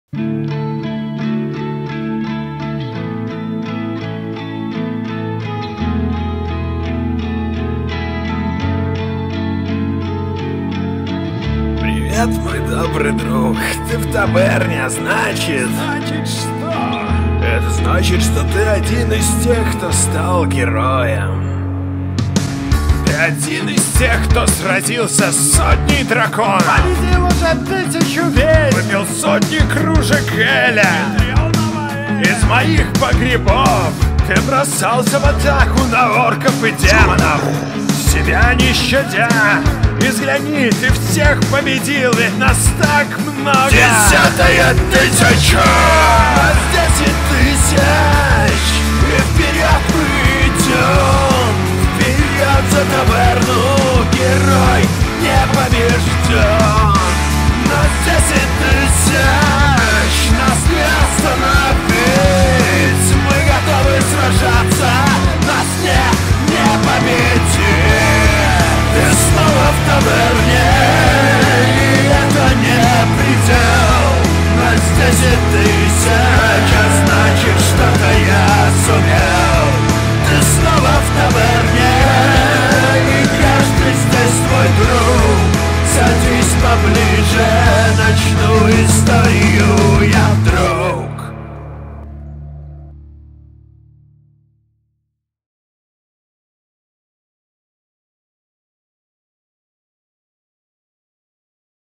10.000 отважных героев Таверны!
10.000 отважных героев Таверны! Я хочу выразить вам свою благодарность за то что вы поддерживаете меня, следите за новостями таверны и просто всегда с нами. Эта небольшая своего рода "песня" - для вас! Потому только благодаря вам и может существовать этот канал! Спасибо друзья мои!